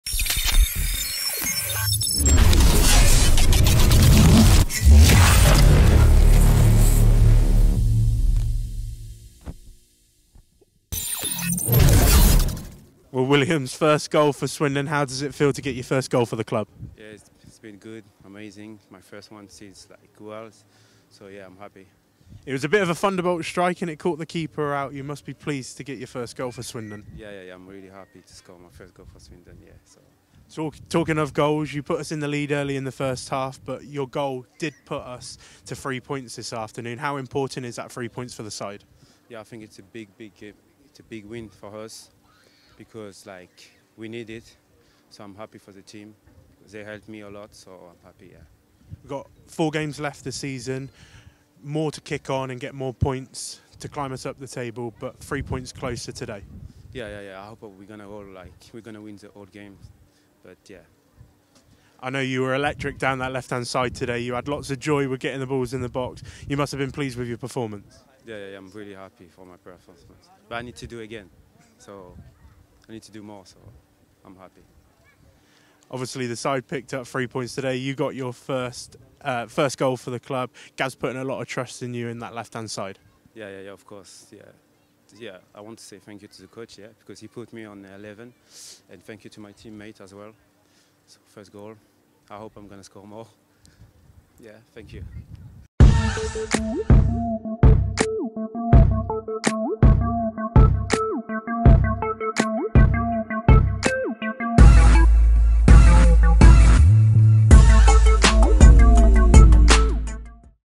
Williams Kokolo on his first goal for the club | Swindon Town Football Club
Defender, Williams Kokolo on his first goal for Swindon Town and important three points this afternoon.